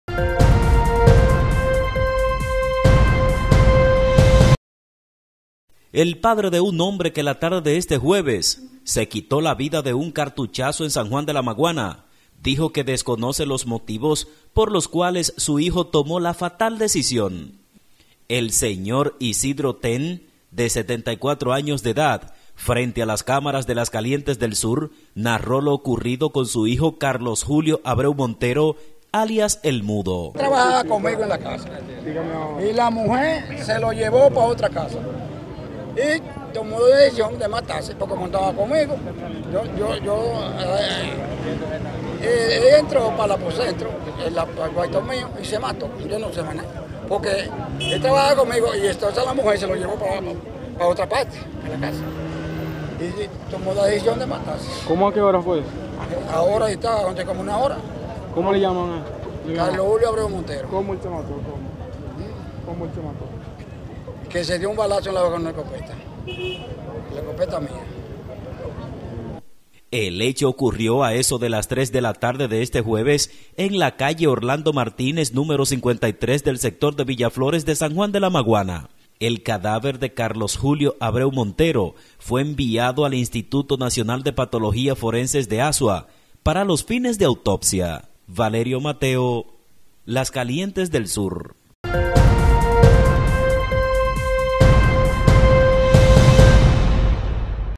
Habla el padre del hombre que se mató de un cartuchazo en San Juan
SAN JUAN DE LA MAGUANA, República Dominicana.- El padre de un hombre que la tarde de este jueves se quitó la vida de un cartuchazo en San Juan de la Maguana, dijo que desconoce los motivos por los cuales su hijo tomó esa fatal decisión.